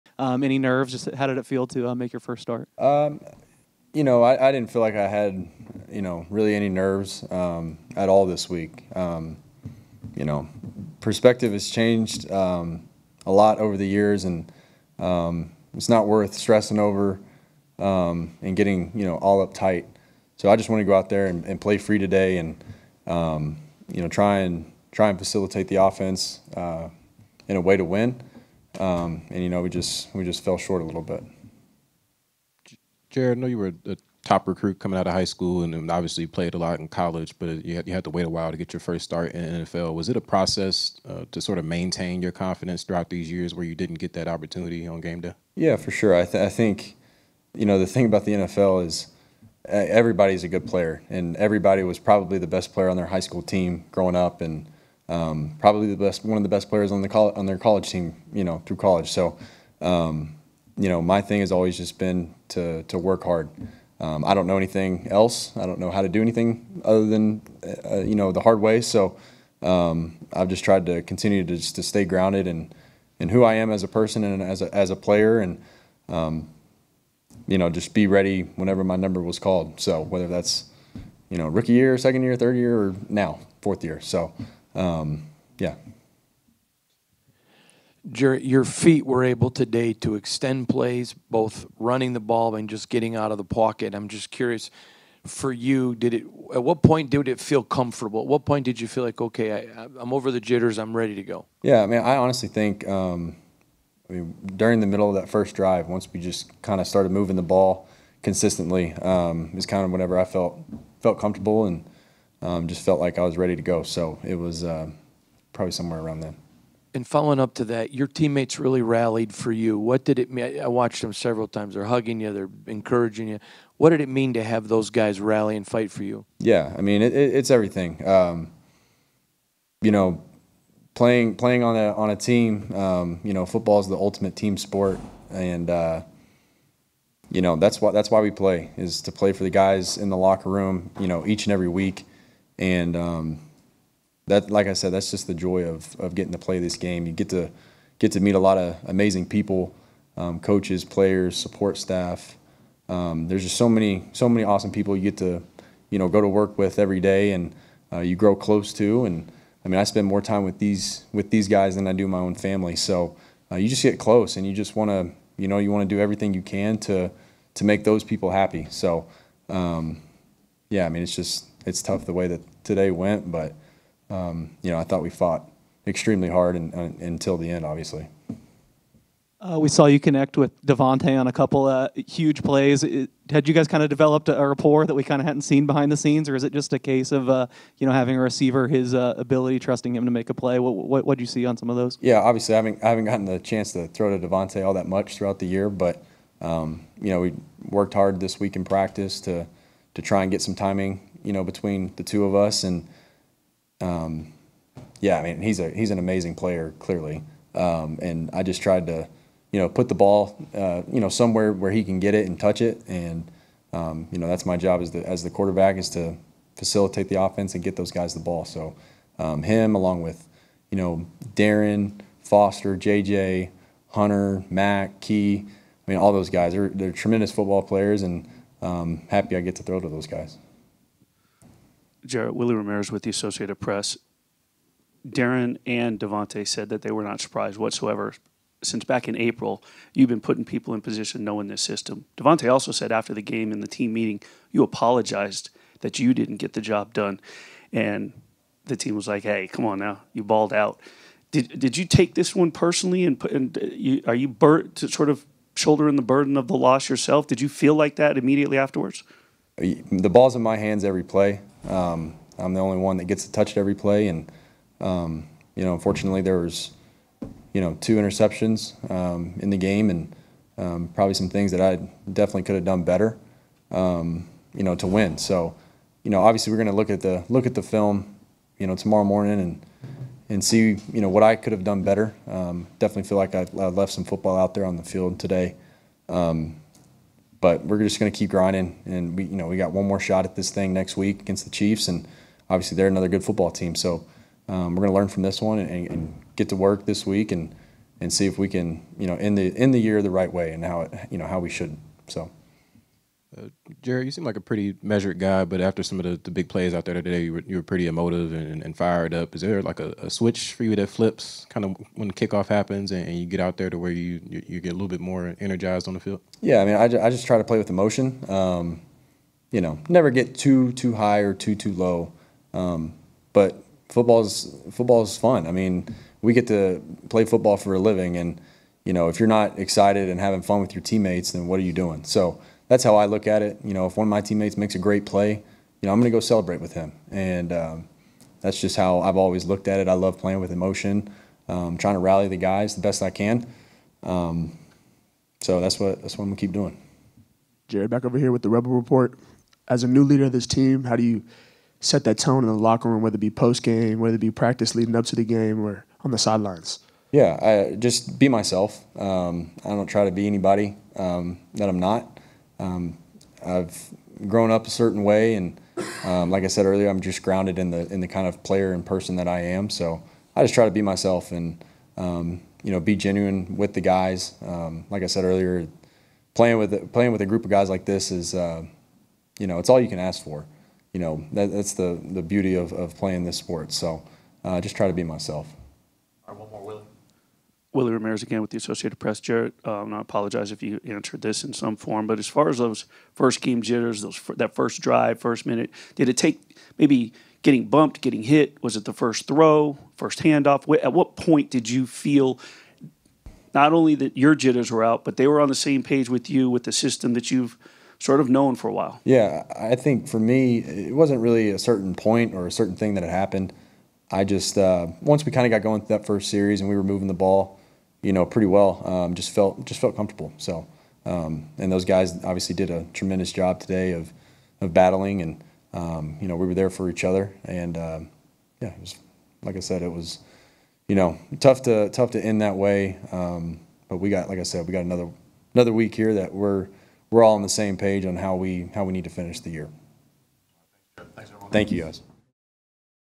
Raiders QB Jarrett Stidham Makes First Career Start
Raiders quarterback Jarrett Stidham spoke to the media following his first career start versus the San Francisco 49ers.